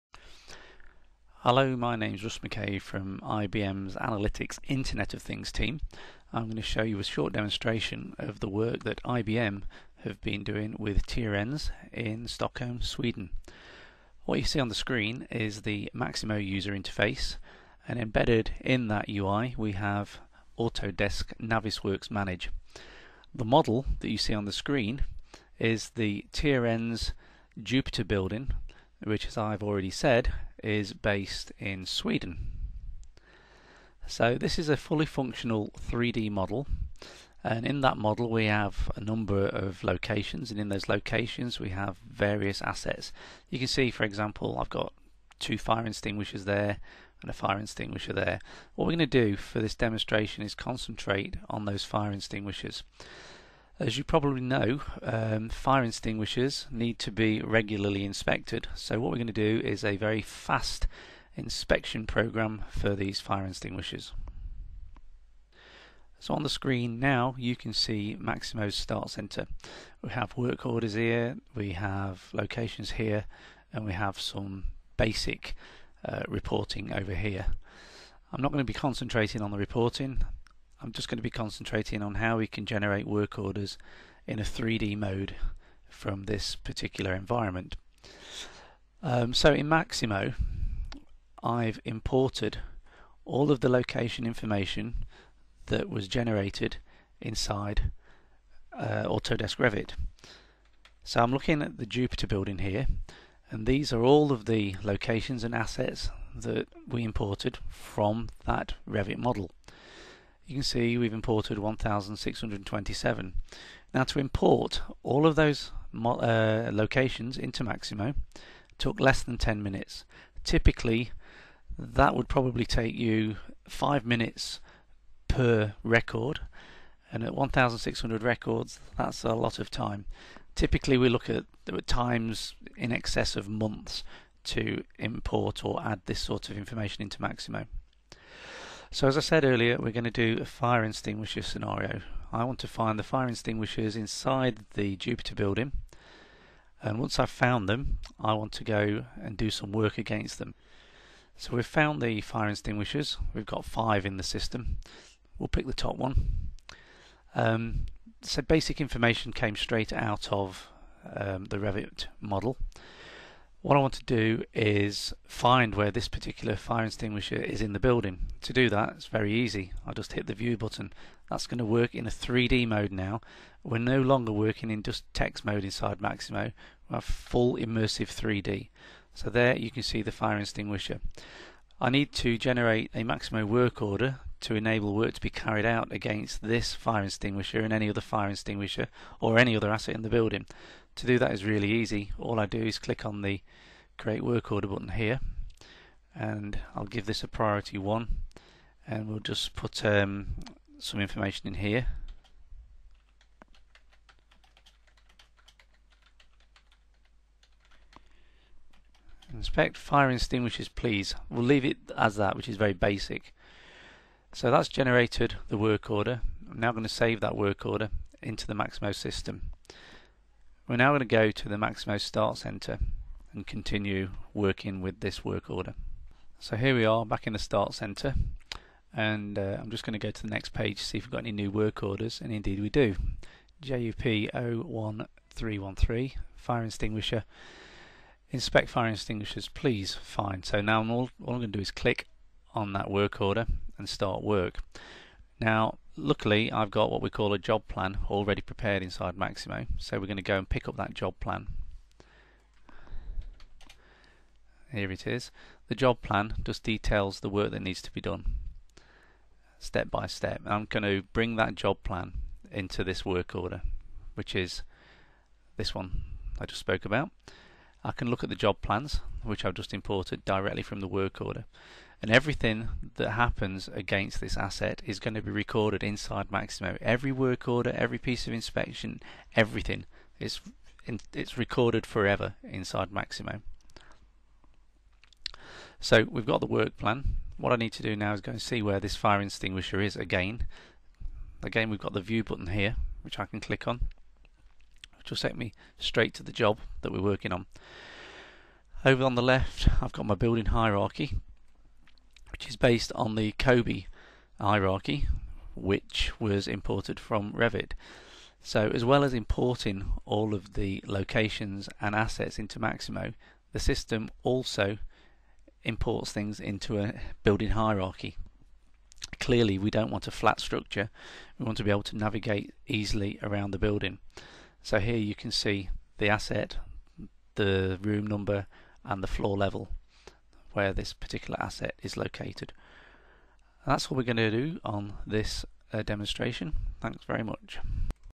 Tyrens BIM demo - Revit och IBM Maximo
Filmen visar ett exempel på hur en Revit-modell kan importeras i IBM Maximo och hur man i Maximo kan nyttja 3D-modellen för att hitta sina förvaltningsbara objekt. Filmen påvisar även nyttan med att BIM ger Maximo en grundläggande informationsstruktur då den importera modellen med en struktur enligt COBie. (Construction Operation Building Information Exchange)